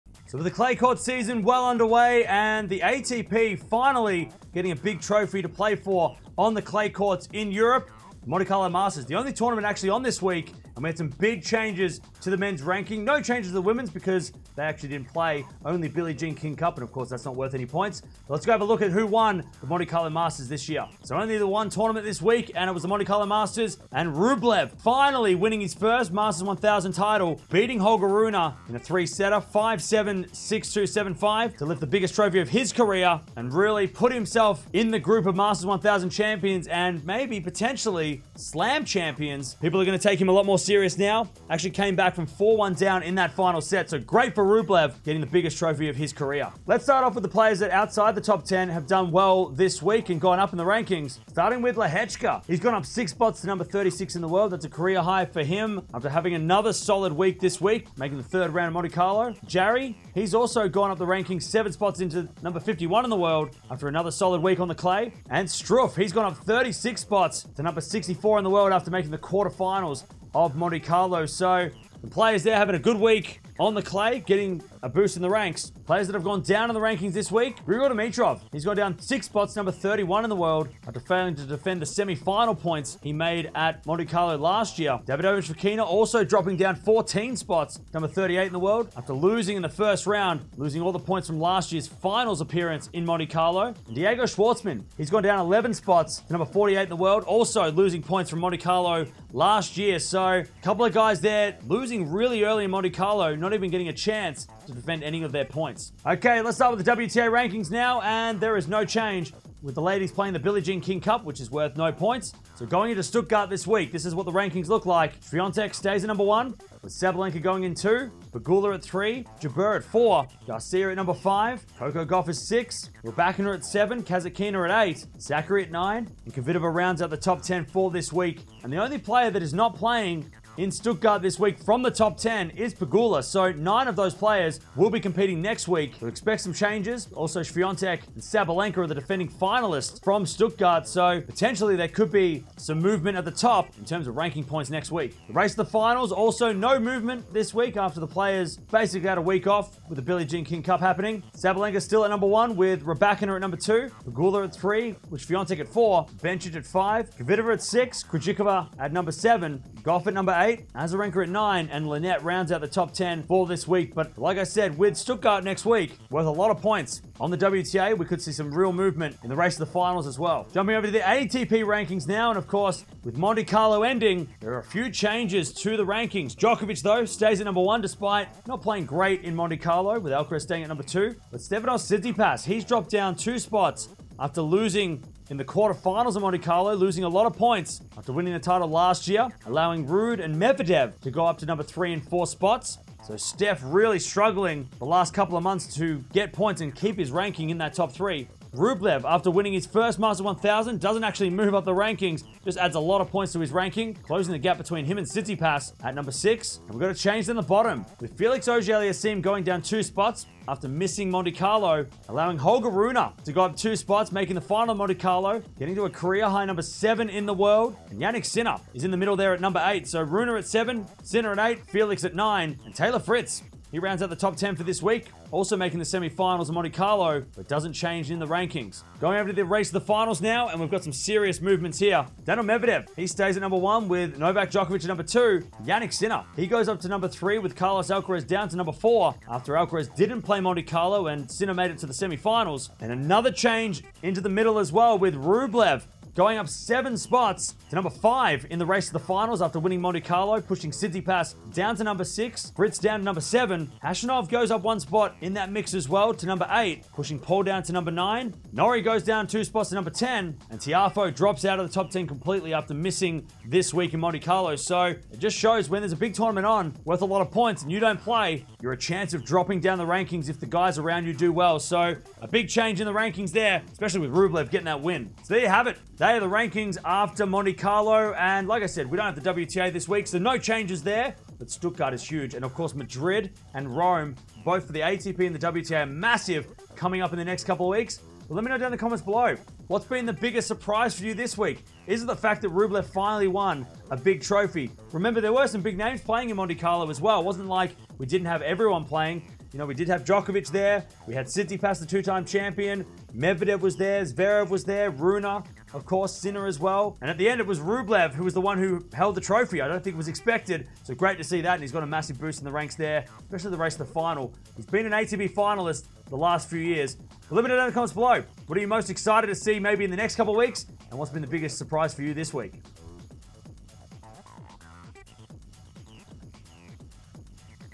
Rublev, Rune, Medvedev Rise | Tsitsipas Falls after Monte Carlo 2023 | Tennis Rankings
With the Monte Carlo Masters done for 2023, Andrey Rublev lifts his first ATP1000 trophy and received a boost up the ranks along with Daniil Medvedev and Holger Rune. Stefanos Tsitsipas fell down the rankings after failing to defend his points from last years event. Who will be favorite going into Stuttgart and Barcelona this week?

0:00 | Intro
0:21 | Past Results
0:46 | Ups & Downs
1:46 | WTA Rankings
2:25 | WTA Finals Race
2:50 | ATP Rankings
3:49 | ATP Finals Race
4:35 | Outro